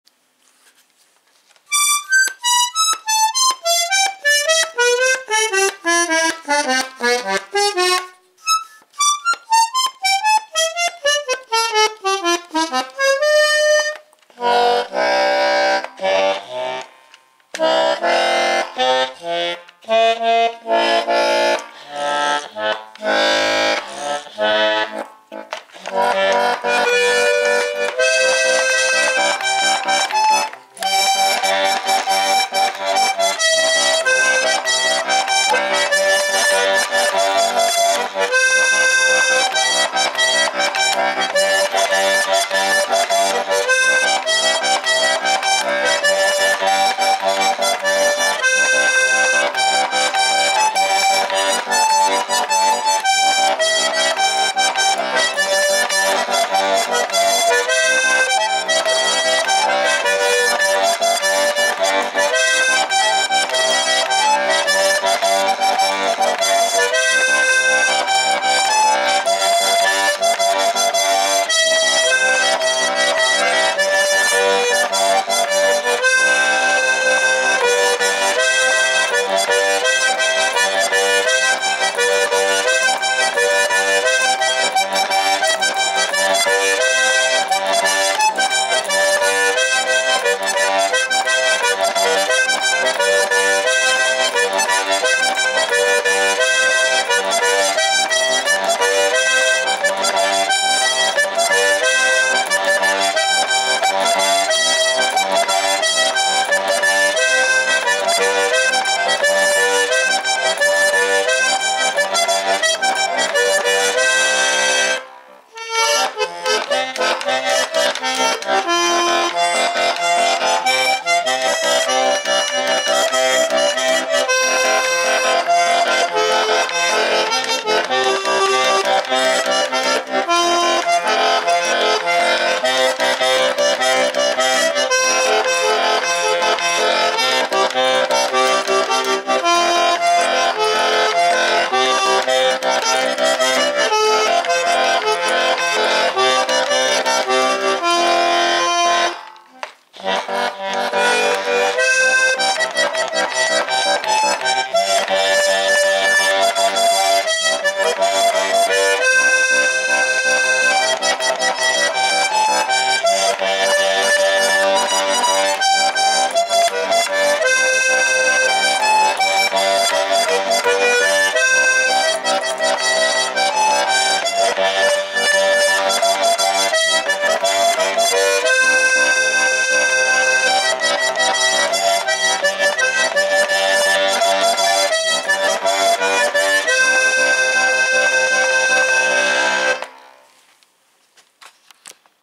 G. LUDWIG 8 bass diatonic button accordion
G. LUDWIG 8 bass diatonic button accordion the key is C/F made in Germany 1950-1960 very rare to find it is an italian model it is hand made beautiful organetto with red wood the box and nice sound bellow is been redone with new dermoide tape and there is a nice design on the bellow and a nice trim comes with one strap and no case will be for sale on Ebay and Reverb.com or by contact me